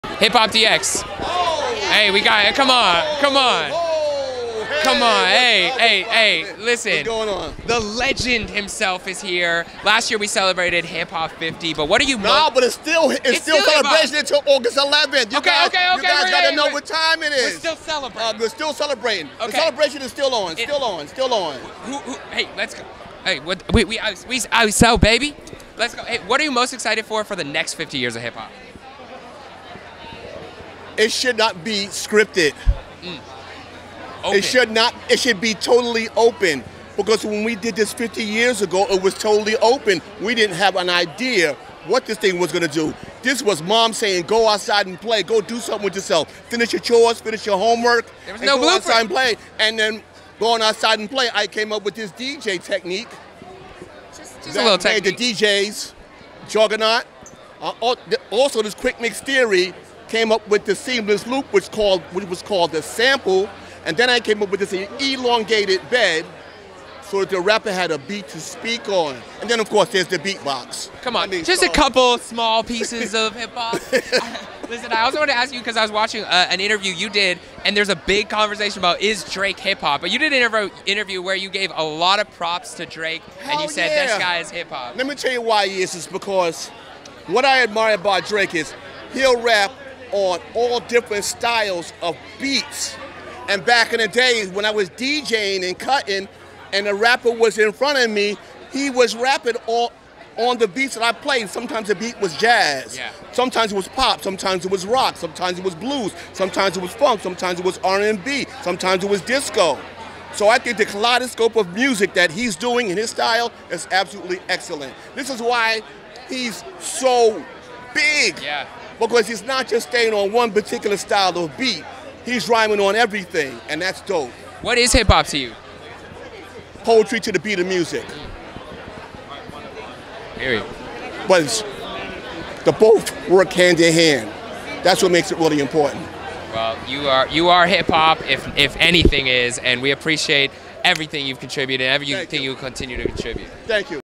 Grandmaster Flash Explains Why Drake Is Hip Hop
We spoke to Grandmaster Flash at the 2024 Grammys about Drake and Mos Def's comments about him not being Hip Hop. He explained why Drake is definitely hip hop

Interview by Jeremy Hecht
Camera operations by Clifton “CT” Tate
Artist Relations by Elise Ricks
Executive produced by Sharath Cherian